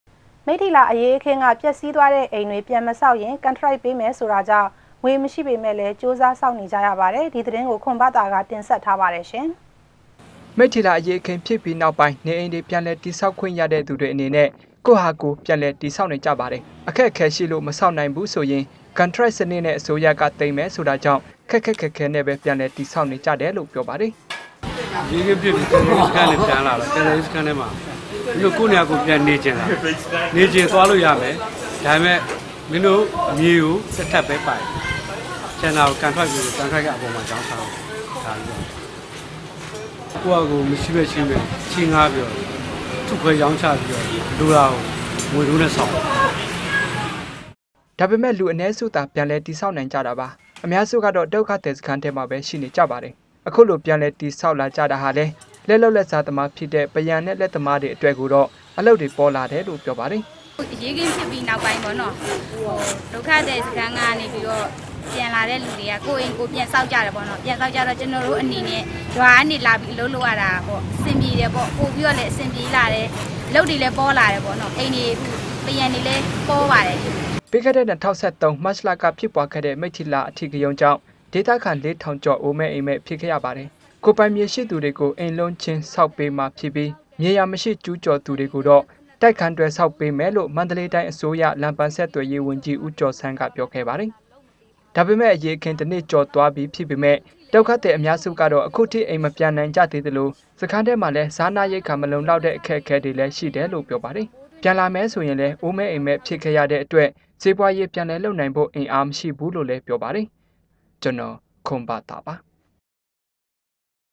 DVB - မိတၳိလာက အိမ္ေတြျပန္မေဆာက္ရင္ ကန္ထရိုုက္ေပးမယ္ဆိုုတာေၾကာင့္
မိတီၳလာ အေရးအခင္းက ပ်က္ဆီးသြားတဲ့ အိမ္ေတြ ျပန္မေဆာက္ရင္ ကန္ထရိုုက္ေပးမယ္ ဆိုုတာေၾကာင့္ ေငြမရွိ ေပမယ့္လည္း ၾကဳိးစား ေဆာက္ေနၾကရပါတယ္။ ဒီသတင္းကို ခြန္ဘသာ ကတင္ဆက္ထားပါတယ္။ ရုုိက္ကူး ေစာနန္း DVB TV - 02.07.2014